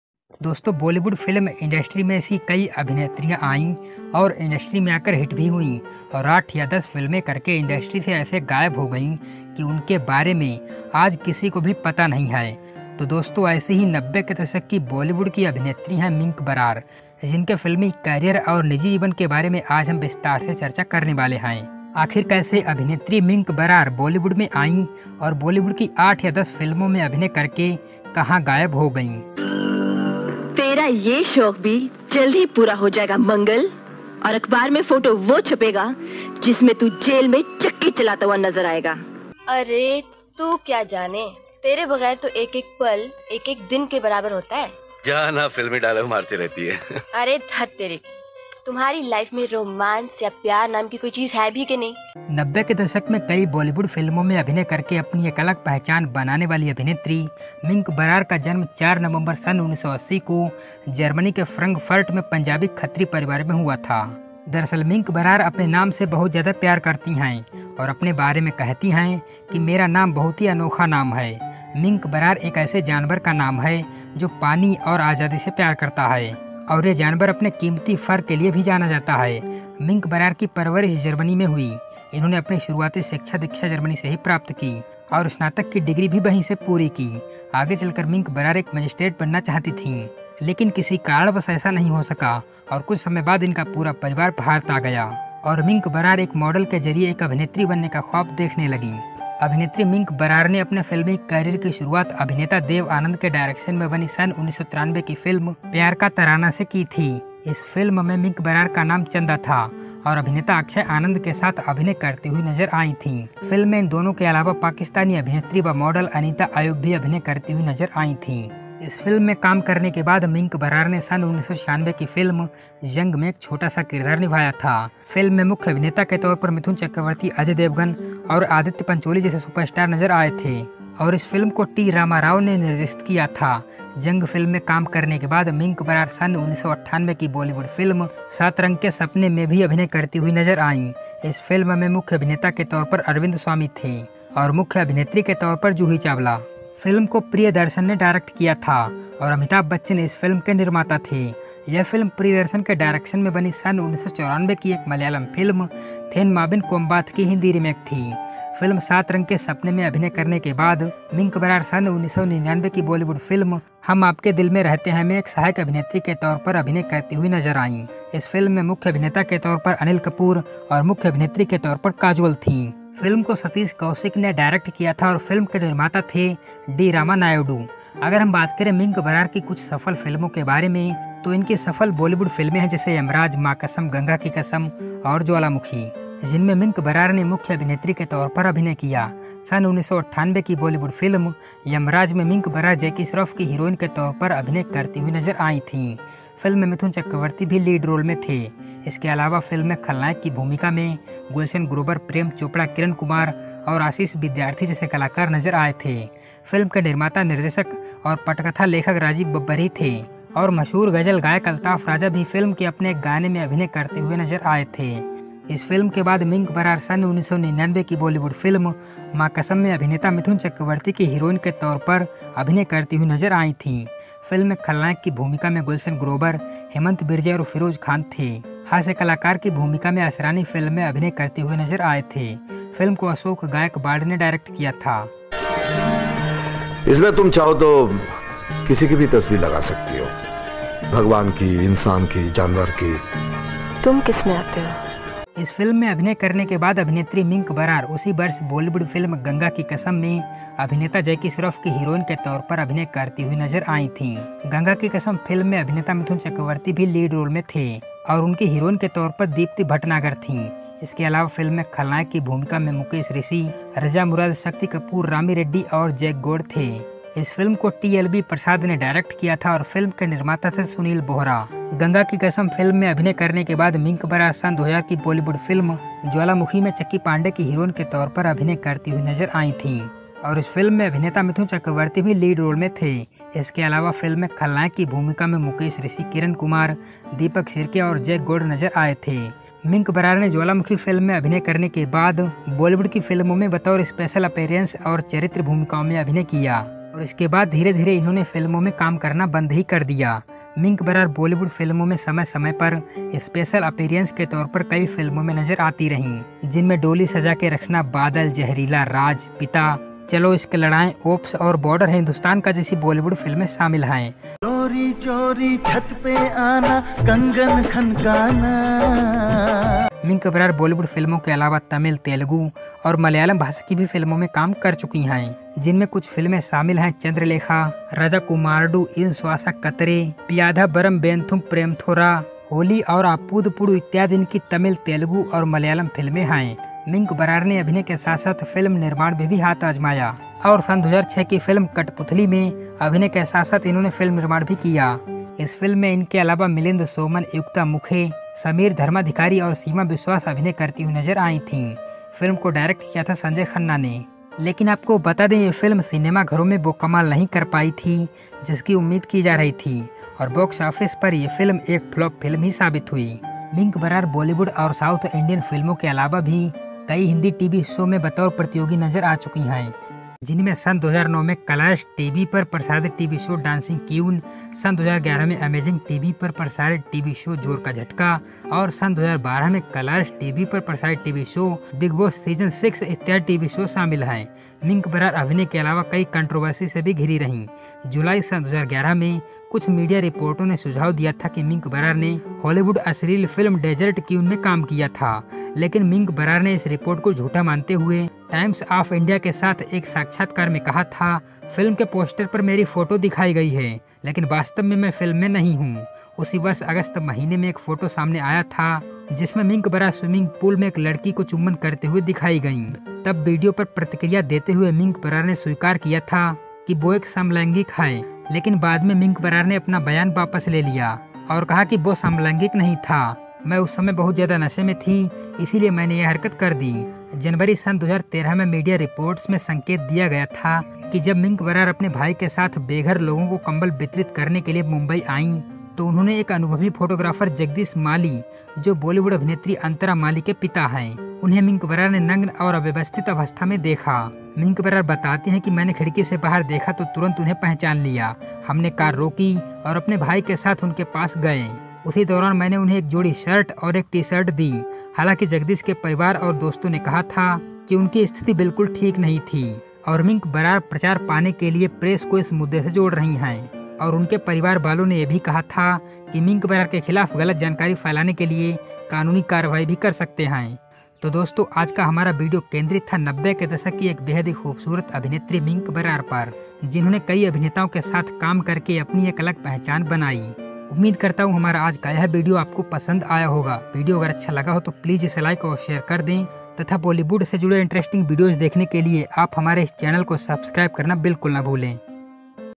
Mink barar biography in Hindi (Aastha Films)
Hi friends

Welcome to Aastha Films.

Mink was born on 4 November1980 in Frankfurt, Germany[ to Punjabi parentswho migrated from India to Germany. About her name, Mink says, "It's a very unique name. Mink is an animal that loves water and freedom, and is known for its precious fur."She was raised in Germany. Mink wanted to be a magistrate as a child. She got her education from Germany and completed a bachelor's degree.

Veteran actor Dev Anand introduced Mink to the Indian film industry when she was 13. She made her debut in the Indian film industry with Dev Anand's Hindi film Pyaar Ka Tarana,which was released in September, 1993. She then appeared in several films like Jung (1996), Saat Rang Ke Sapne (1998), Yamraaj (1998), Hum Aapke Dil Mein Rehte Hain (1999), Ganga Ki Kasam (1999), Maa Kasam (1999), Jwalamukhi (2000), Ajnabee (2001), Zahreela (2001), Pitaah (2002), Chalo Ishq Ladaaye (2002), Raaz(2002),Border Hindustan Ka (2003), and Oops (2003). She also worked in some regional South Indian films, including a Telugu movie, Prematho Raa (2001)

Mink was raised in a conservative and protective environment, with her family trying to inculcate in her traditional Indian values instead of western mores.Her immediate family consists of her mother and brother named Punnu Brar, who is three years older to her.